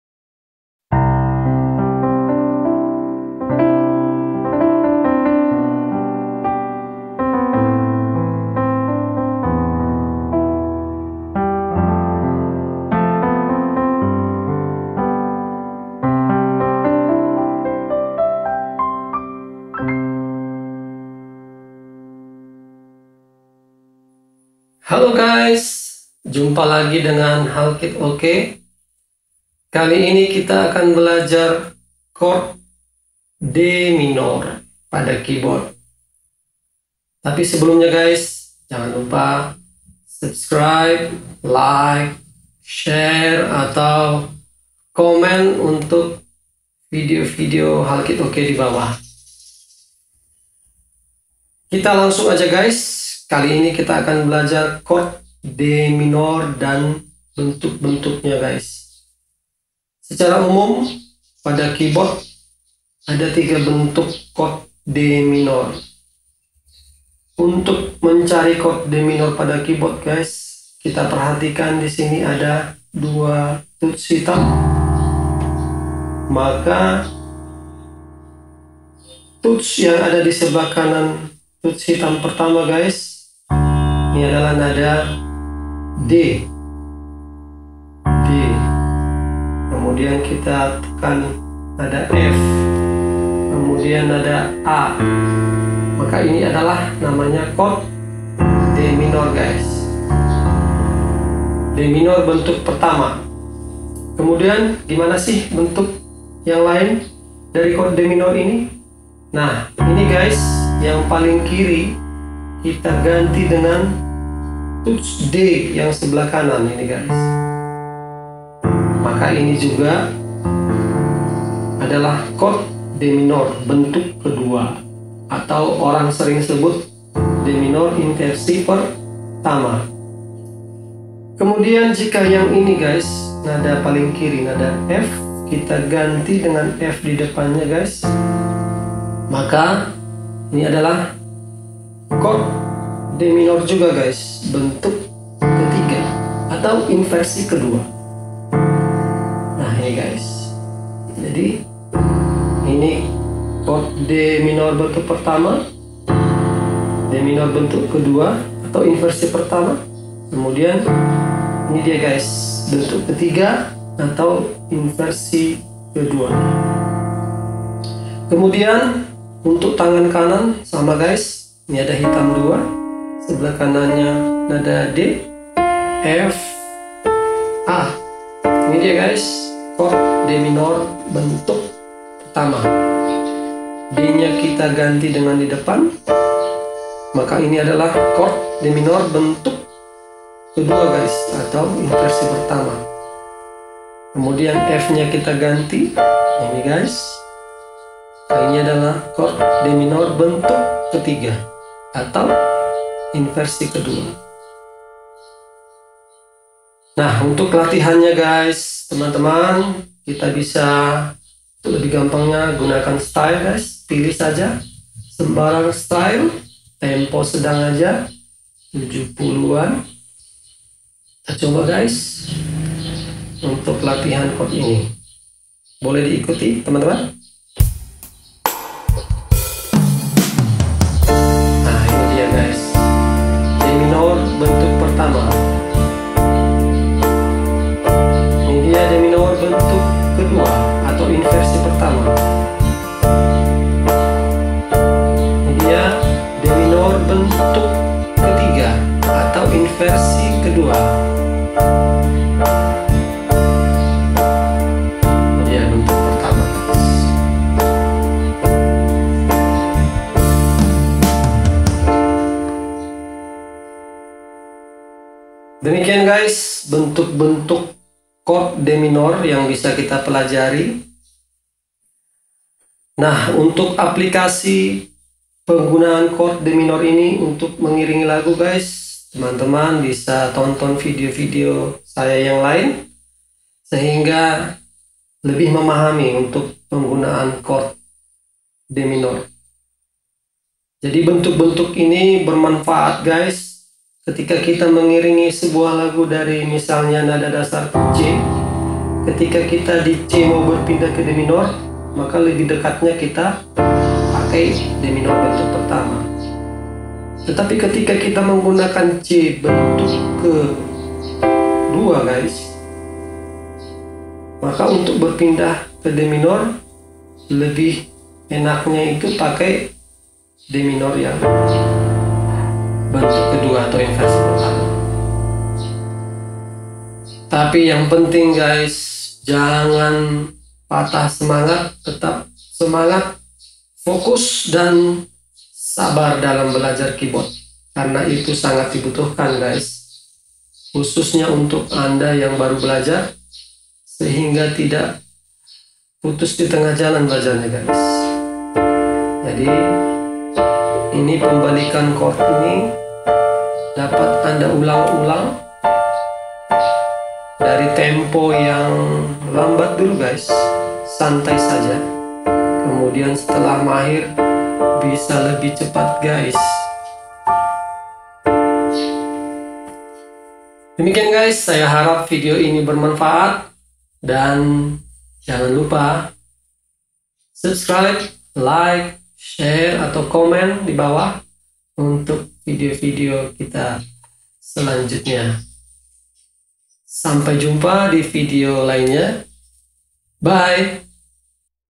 BELAJAR PIANO PEMULA | Chord D Minor
Cara mudah belajar keyboard sendiri. Mari belajar chord piano dan keyboard untuk dapat mengiringi lagu-lagu favorit kita. Disajikan dengan cara yang mudah dimengerti, video ini akan membimbing anda sebagai pemula untuk belajar kunci keyboard lengkap.